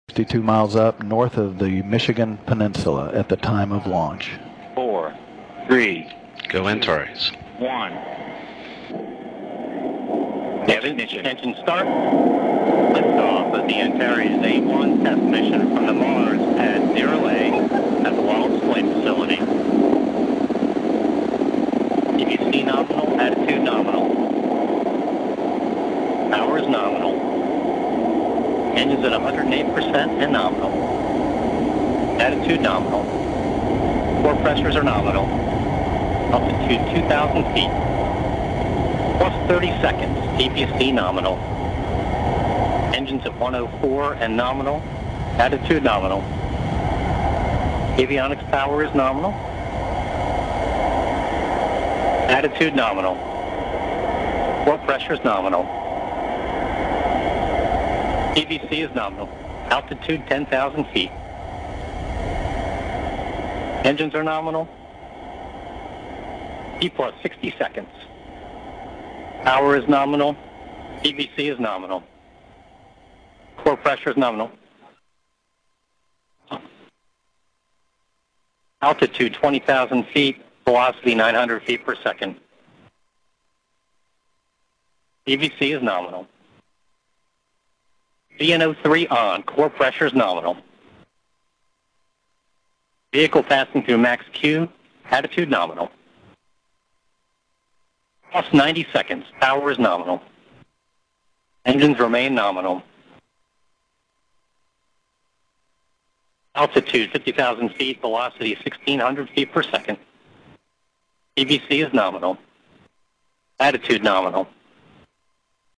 Первый запуск «Антареса» \ First launch of Antares
First launch of Antares rocket
22 апреля с космодрома на атлантическом побережье США была впервые запущена ракета «Антарес»
Прямая трансляция пуска ракеты велась на сайте NASA